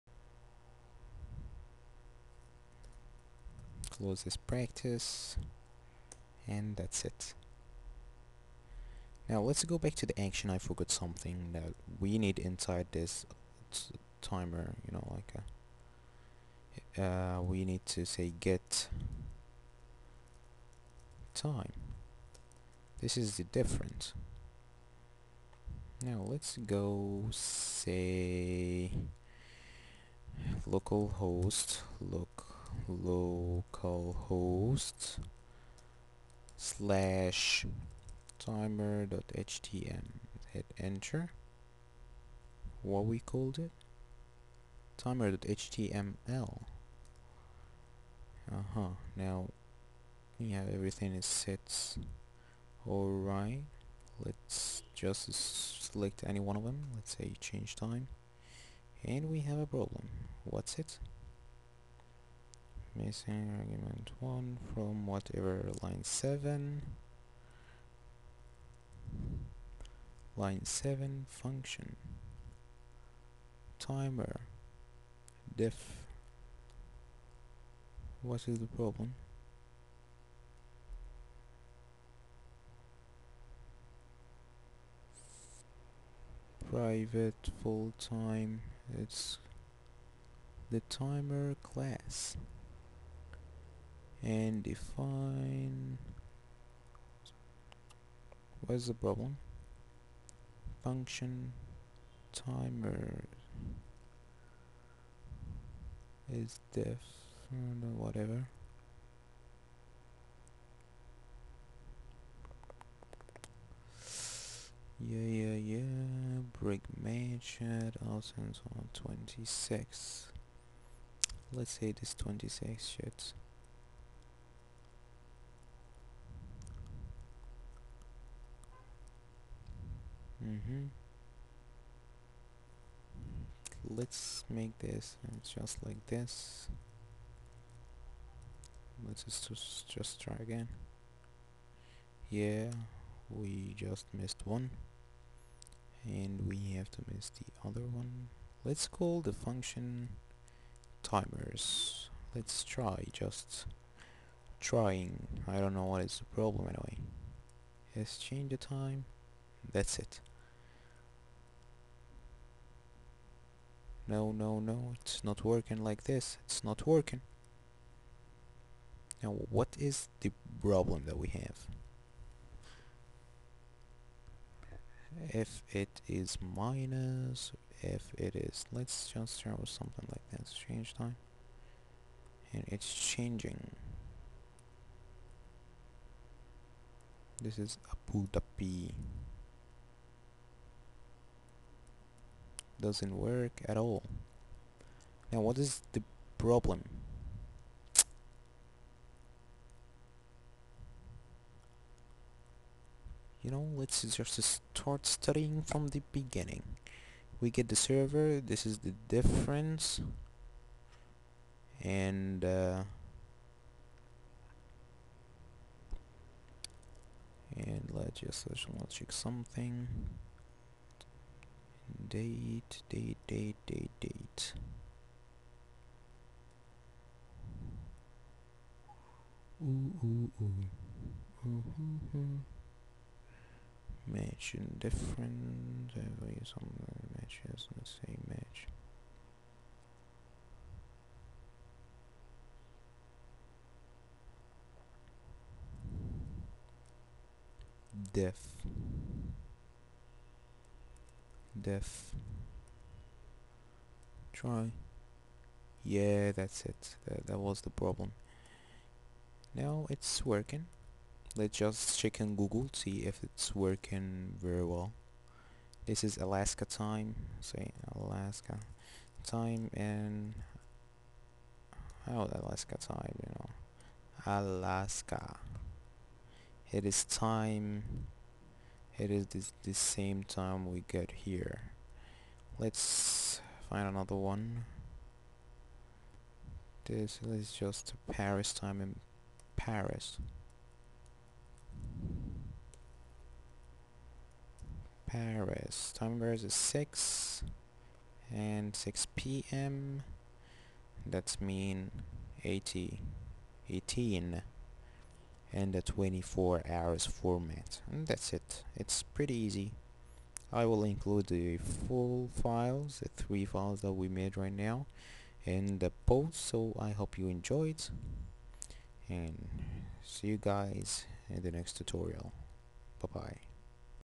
PHP Tutorial: Timezone Changer Part 2
I'm going to make php timezone changer... and will use some basic ajax to return result... Checkout the Full Post @